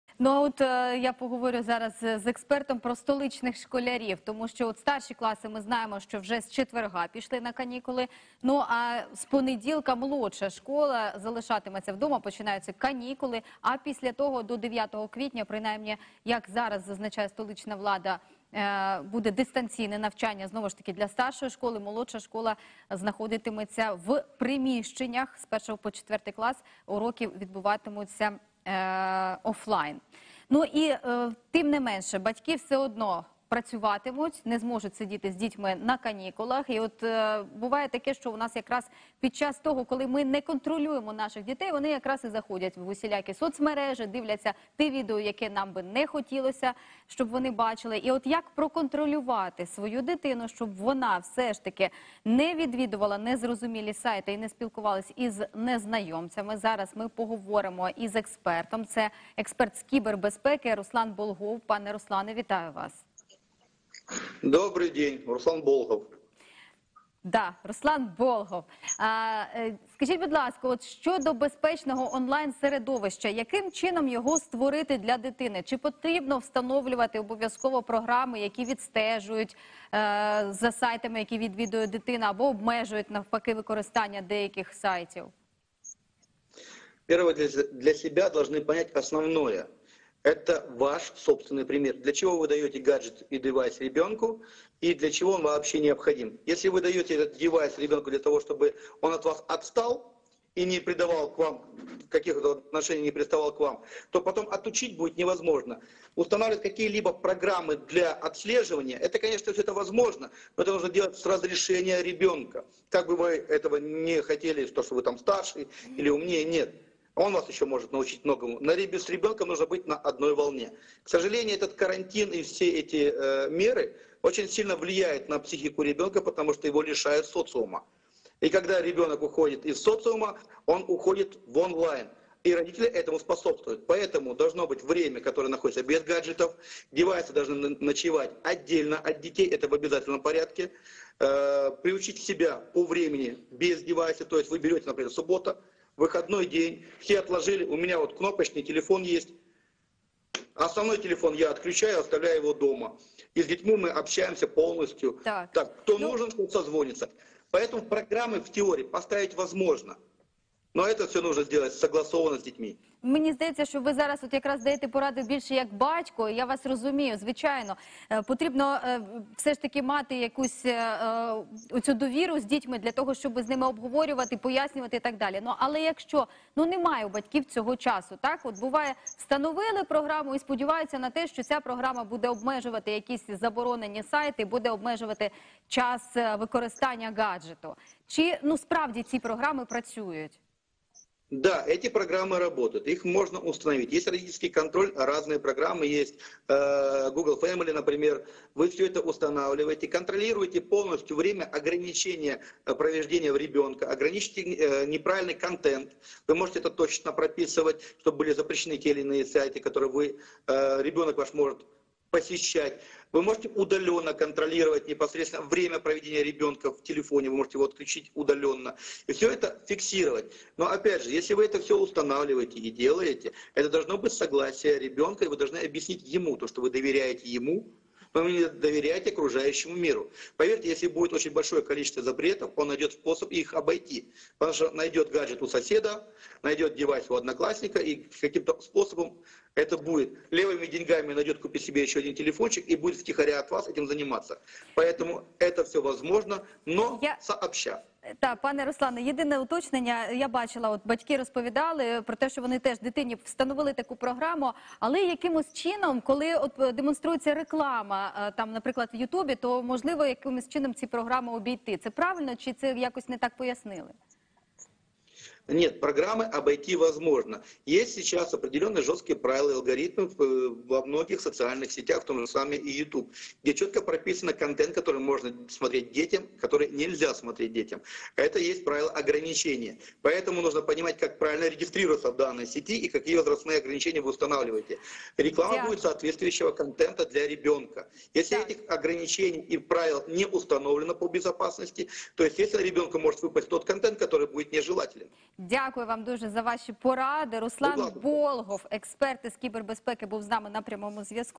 Як створити для дітей безпечне онлайн-середовище?
Незалежно від того, скільки часу дитина проводить онлайн, вона може натрапити на небезпечні матеріали: порнографію, сцени ненависті чи насильства або стати жертвою цькувань.